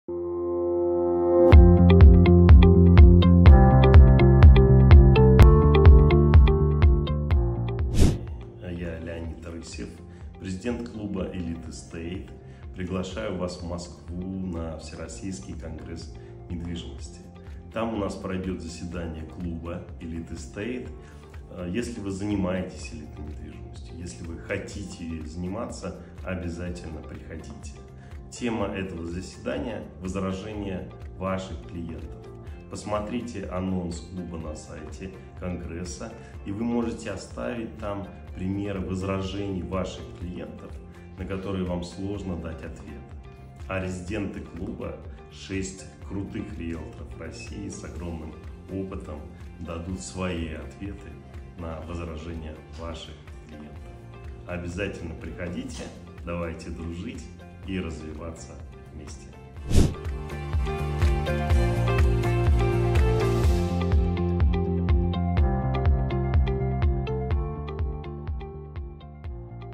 Приглашение от Леонида Рысева на заседание Клуба брокеров Elite Estate
Уважаемые коллеги!

Леонид Рысев, президент Клуба брокеров
Elite Estate, генеральный директор компании «Элитные квартиры» приглашает посетить заседание клуба.

Заседание Клуба брокеров Elite Estate пройдет 16 октября, в зале «ГК Точно» с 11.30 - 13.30.

В клубе Elite Estate вы сможете обмениваться опытом и обсуждать стратегии работы с элитной недвижимостью. Заседания нашего клуба посвящены таким темам, как развитие личного бренда, работа с возражениями и другие.